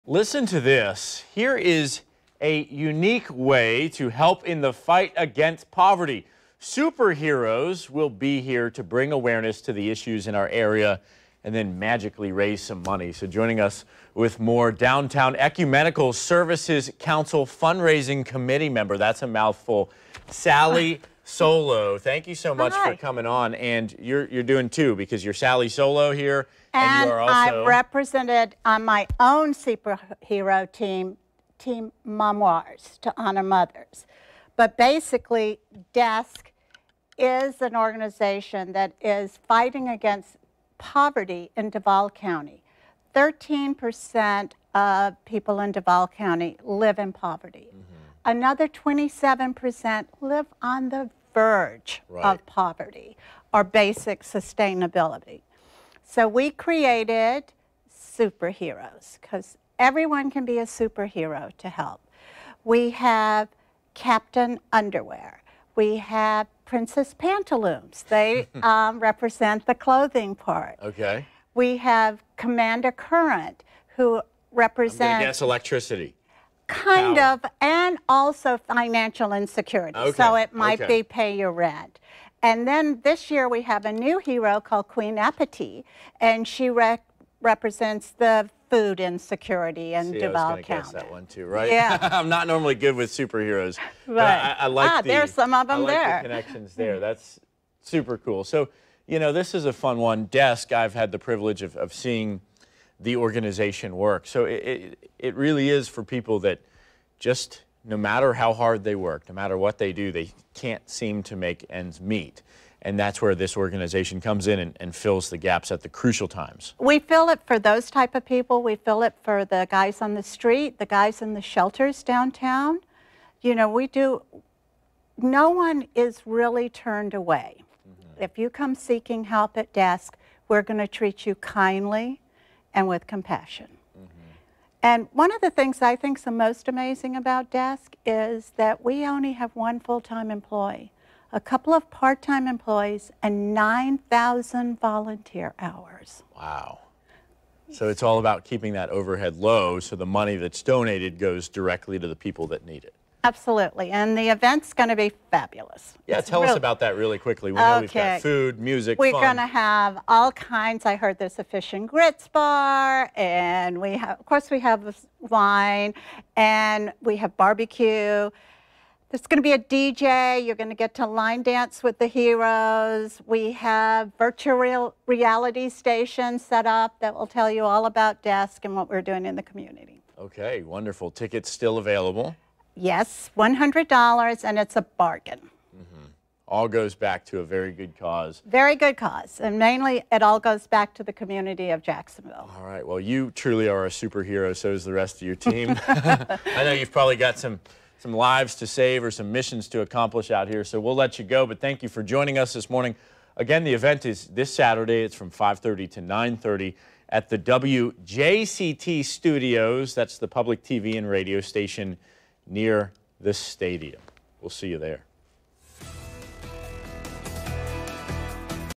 Defenders superhero challenge
Salli Solow joins us with a way you can help in the fight against poverty.